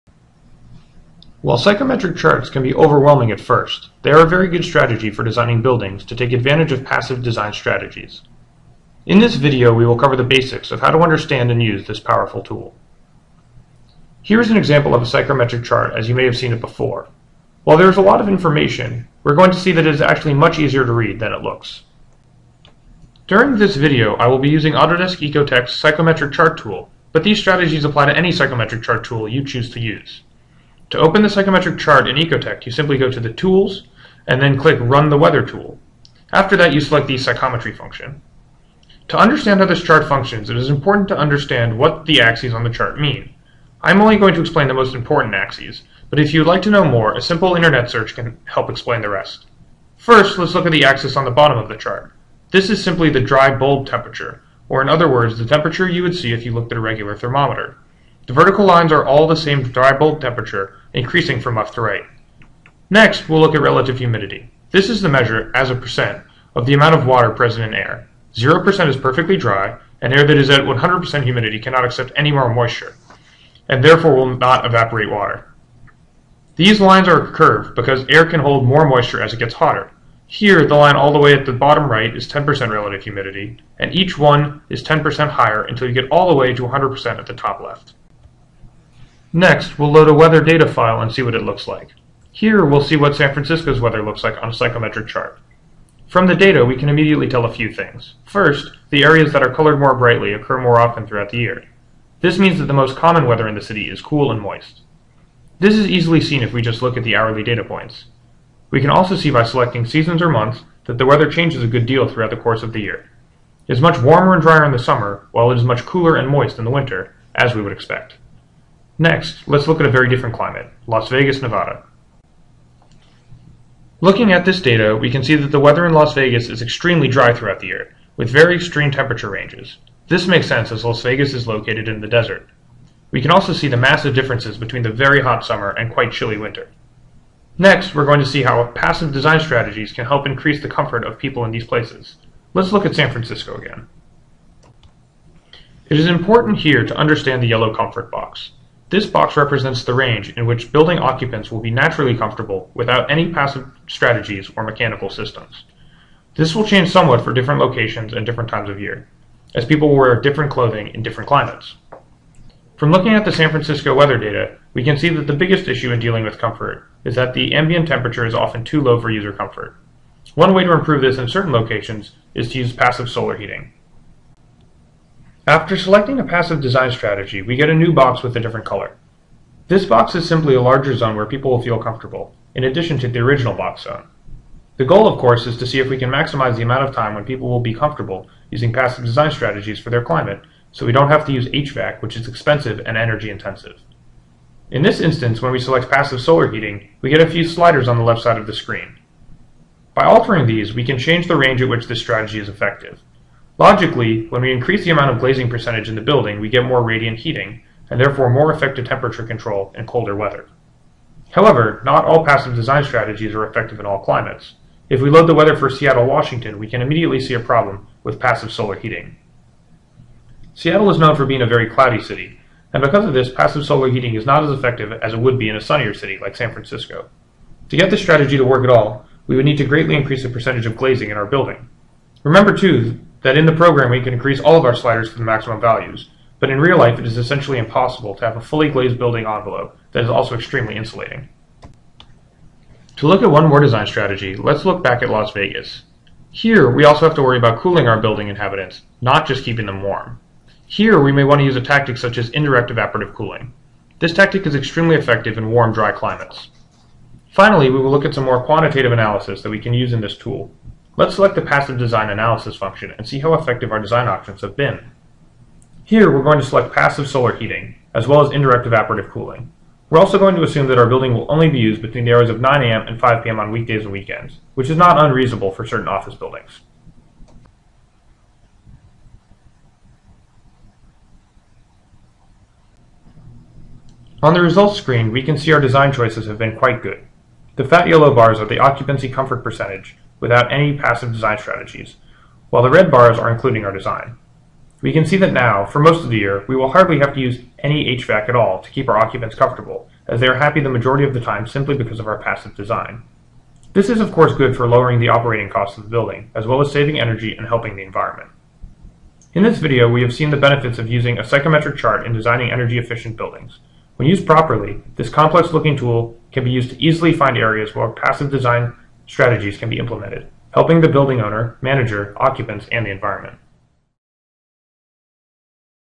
Using Psychrometric Charts for Building Design
This video covers the basics of understanding psychrometric charts, as well as how to use them for effective passive design in new and retrofitted buildings.